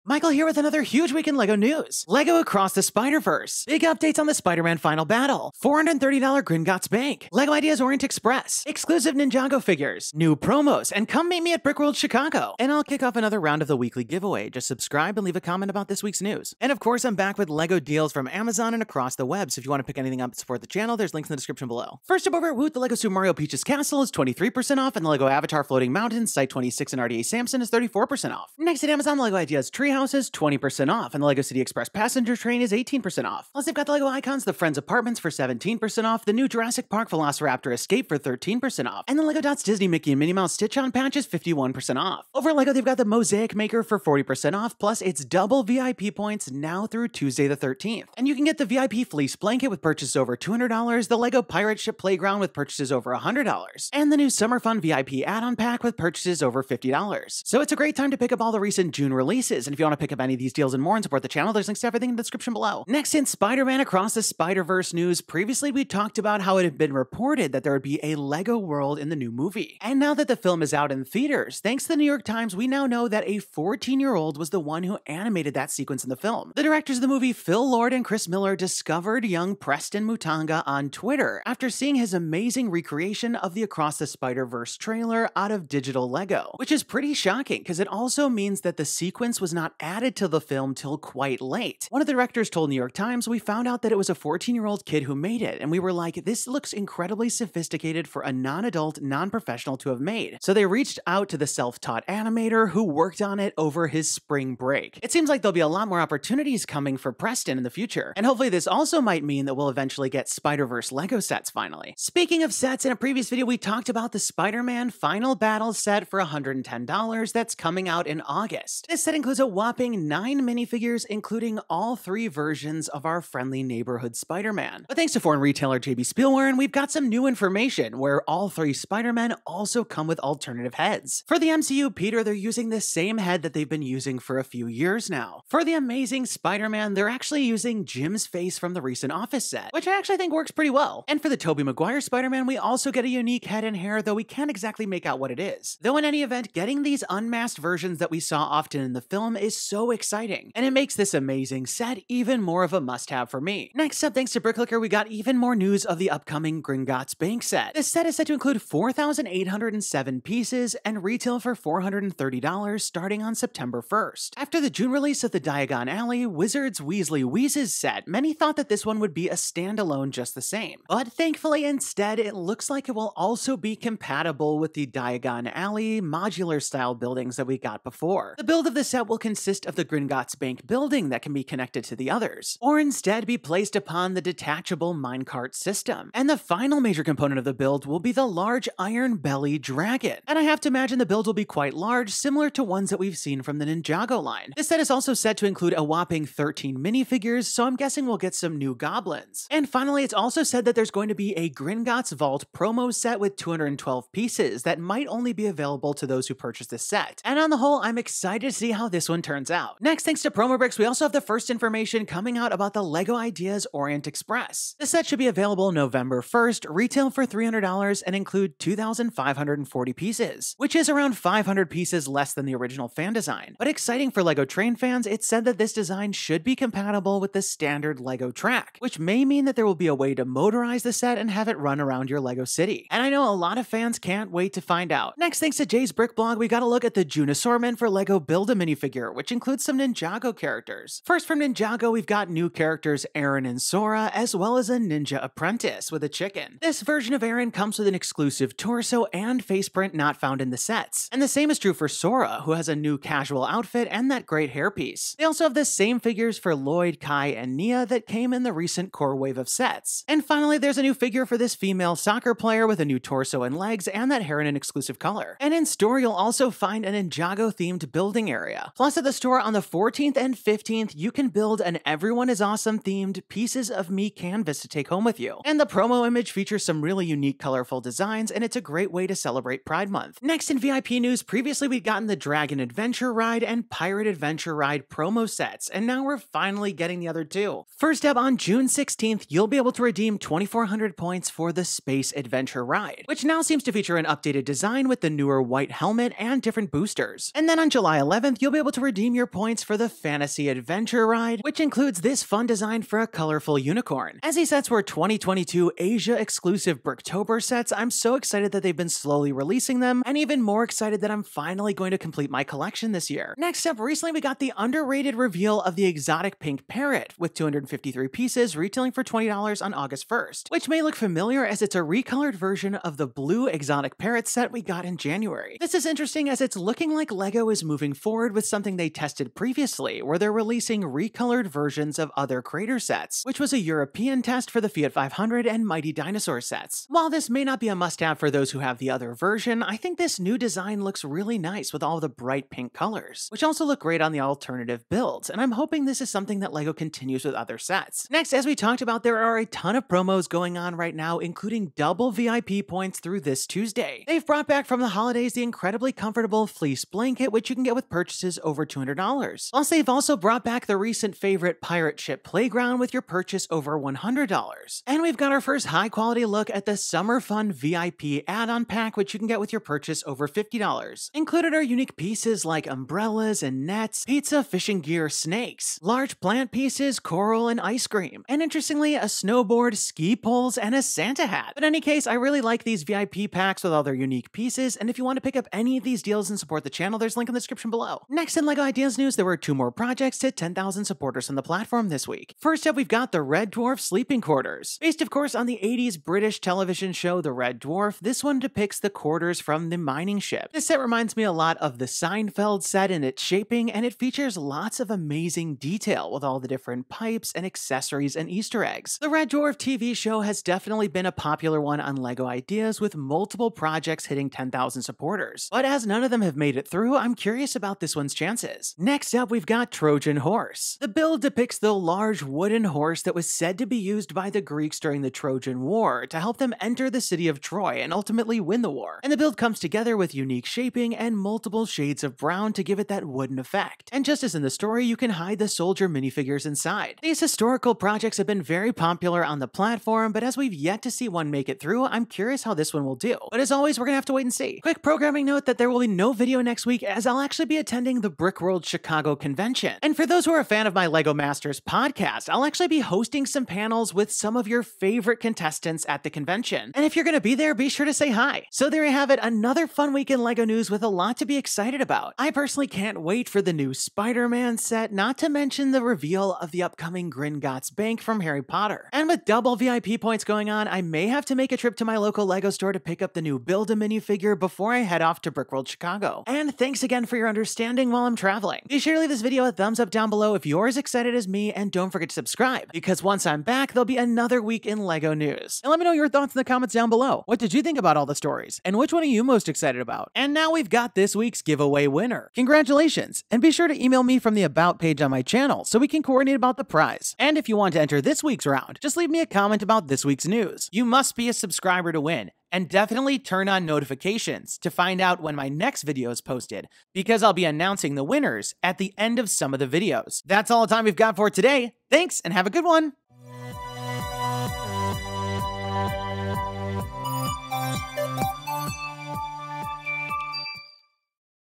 LEGO NEWS! LEGO Spider-Verse?! Spider-Man Minifigs! $430 Gringotts! Ideas Orient Express! New Promos
Welcome to TalkBricks!

Today I'm breaking down This Week in LEGO NEWS!!

LEGO -DOUBLE VIP POINTS! FREE Pirate Playground! FREE Summer VIP Add-On! FREE VIP Blanket!
- LEGO DC Batcave – Shadow Box
- LEGO Icons PAC-MAN Arcade
- LEGO NINJAGO City Markets
- Up-Scaled LEGO Minifigure
- LEGO Disney Villain Icons

LEGO DEALS!!
- WOOT! LEGO Deals!
- LEGO Super Mario Peach’s Castle (23%)
- LEGO Avatar Floating Mountains: Site 26 & RDA Samson (34%)
- LEGO Mosaic Maker (40%)
- LEGO Ideas Tree House (20%)
- LEGO Jurassic Park Velociraptor Escape (13%)
- LEGO Icons The Friends Apartments (17%)
- LEGO City Express Passenger Train (18%)
- LEGO DOTS Disney Mickey and Minnie Mouse Stitch-On Patch (51%)
- LEGO Super Mario Cat Peach Suit and Frozen Tower (31%)
- LEGO Everyone is Awesome (30%)
- LEGO Marvel Miles Morales vs. Morbius (20%)
- LEGO Jurassic World T. rex & Atrociraptor Dinosaur Breakout (30%)
- LEGO Black Panther Bust (40%)
- LEGO Classic Creative Fantasy Universe (30%)
- LEGO City Farmers Market Van (39%)
- LEGO Minecraft The Bakery (32%)

14 Year Old LEGO Animator on Spider-Man Across the Spider-Verse

LEGO Spider-Man Final Battle New Details!

LEGO Harry Potter Gringotts Bank (76417) Coming September 1st!
- 4807 pieces - $429.99
- Includes 13 minifigures
- Gringotts Vault (40598) Promo - 212 pieces

LEGO Ideas Orient Express (21344) Coming November 1st!

Final Two LEGO Bricktober 2022 Sets Coming Soon to Rewards Center
- Space Adventure Ride: June 16
- Fantasy Adventure Ride: July 11

June 2023 LEGO Store Ninjago Build-a-Minifigure (BAM) Minifigures Now Available!

LEGO Store Build a Mini - Pieces of Me Canvas Event!
- 6/14 and 6/15 between 5-7PM

LEGO Creator 3-in-1 Exotic Pink Parrot (31144) Coming this August!
- 253 pieces - $19.99

00:00 - Intro
00:16 - LEGO Deals!
01:02 - LEGO Across the Spider-Verse! 14yo Animator?!
01:51 - LEGO Spider-Man Final Battle Updates!
02:33 - LEGO Harry Potter $430 Gringotts Updates!
03:29 - LEGO Ideas Orient Express Set Details!
03:55 - June LEGO Store BAM Ninjago Minifigs!
04:25 - LEGO Store InStore Events!
04:40 - LEGO VIP Space & Fantasy Adventure Rides!
05:13 - LEGO Creator Exotic Pink Parrot!
05:48 - LEGO June Promos & GWPs!
06:28 - More Projects Hit 10K on LEGO Ideas!
07:24 - Meet Me at Brickworld Chicago 2023!
07:38 - Outro

TalkBricks with us!